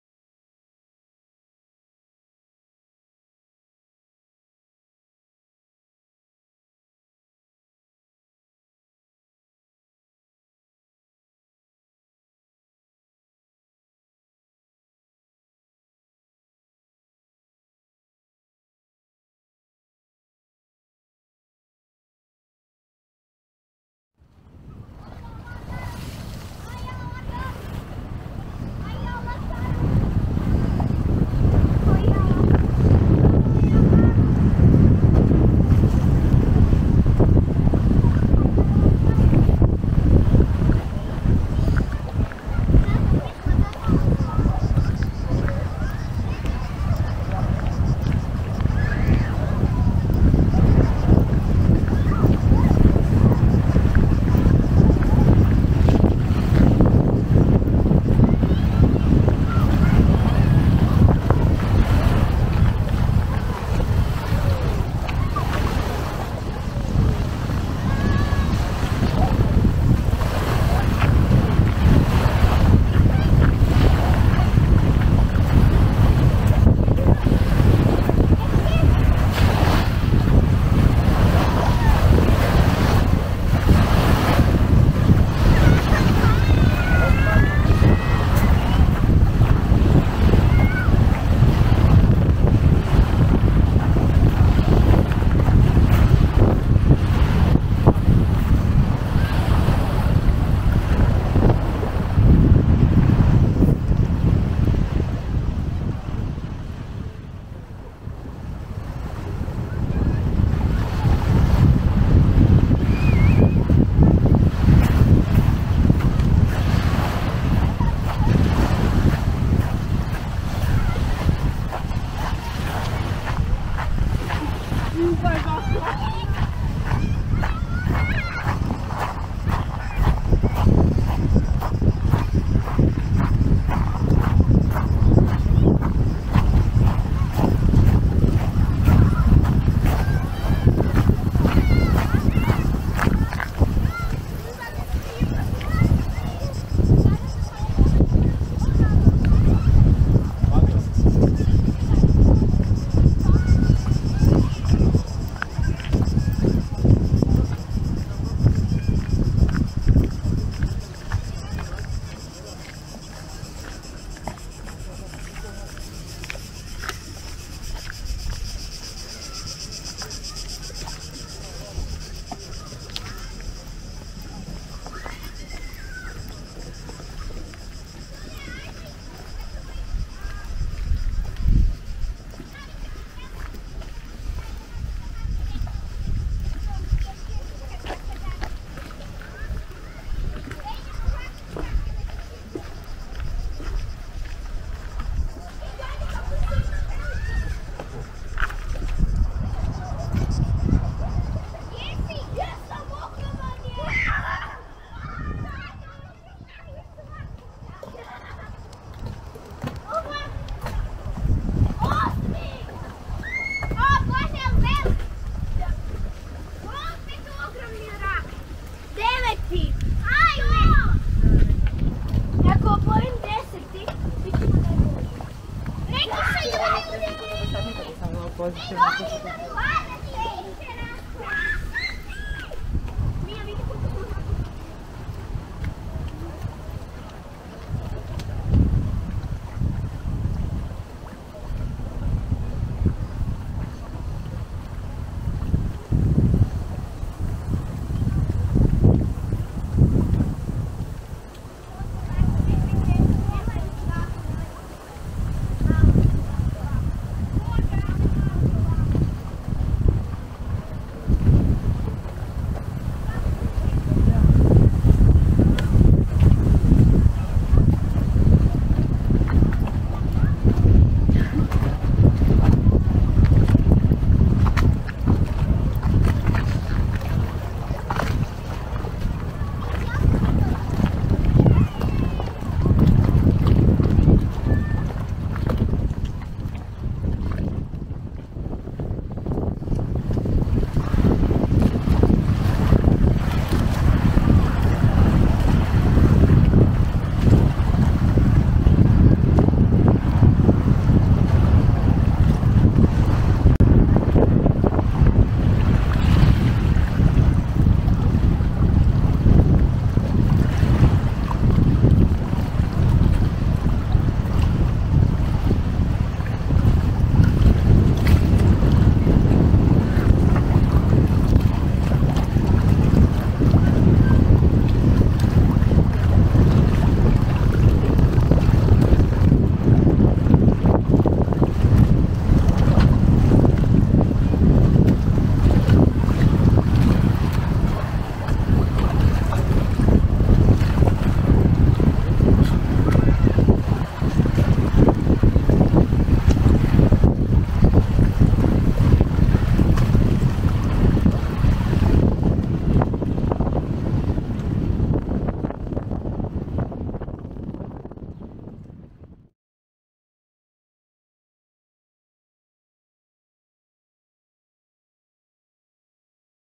Small Marina in Brodarica - Walking Tour | Croatia | Dalmatia [2022 Aug.]
In this video we are still in Brodarica town in the middle of Dalmatia (Croatia).
My previous video presented Brodarica town beach.
This one is taking us to the small marina where you can catch the ferry straight to the Krapanj Island situated 600 metres from the land. It is very small marina but very  charming and calming to everybody who wants to spend there some time :-)